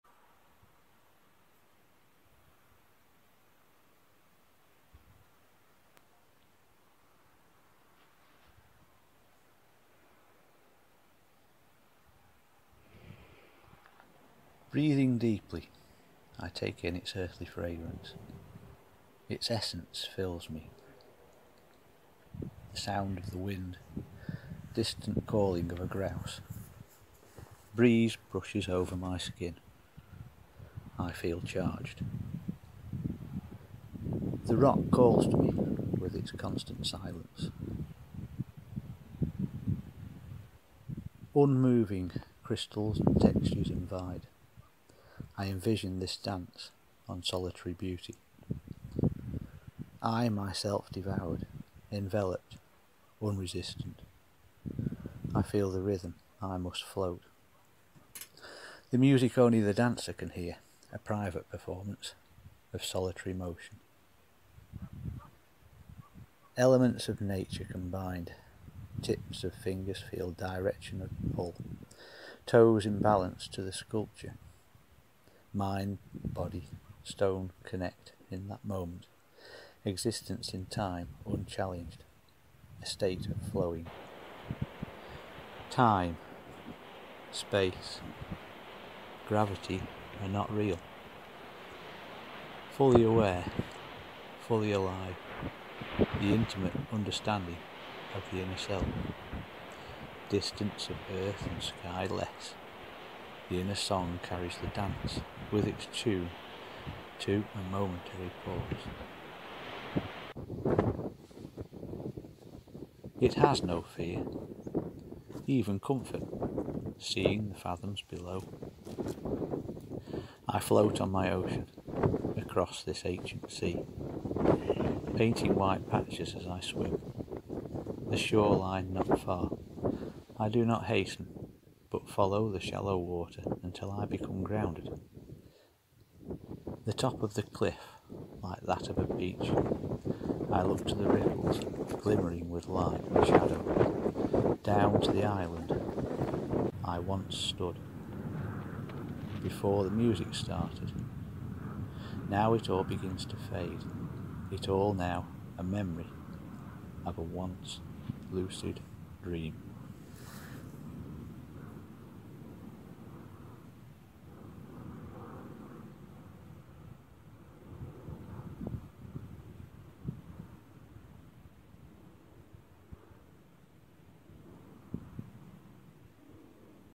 solitary motion ( expression in climbing)
a very personal account of what climbing means to me .A connection to nature , by the moment,  to live and breath and be totally given to one point fully. Fully alive fully aware , ONE

solitary motion ( by R Taylor )

breathing deeply
I take in its earthly fragrance
it's essence fills me
the sound of the wind
distant calling of a grouse
breeze brushes over my skin
I feel charged
the rock calls to me with it's constant silence
unmoving
crystals and textures  invite
I envision this dance on stationary  beauty
I myself  devoured
enveloped
unresistant
I feel the rhythm I must float
the music only the dancer can hear
a private performance
of solitary motion
elements of nature combine
tips of fingers  feel direction of pull
toes in balance to the sculpture
mind
body
stone
connect in that moment
existence  in time unchallenged
a state of flowing
time
space
gravity
are not real
fully aware
fully alive
the intamate understanding of the inner self
distance of earth and sky less
the inner song carries the dance with it's tune to a momentary  pause ,, ,,
it has no fear
even comfort
seeing the fathoms below
I float on my ocean
painting white patches as I swim
the shore line not far
I do not hasten
but follow the shallow water until I become grounded
the top of the cliff
like that of a beach
l look to the glimmering ripples of light and shadow
down to the island
I once stood
before the music started
now it begins to fade ,,  ,,,,
it all now a memory of a once lucid dream .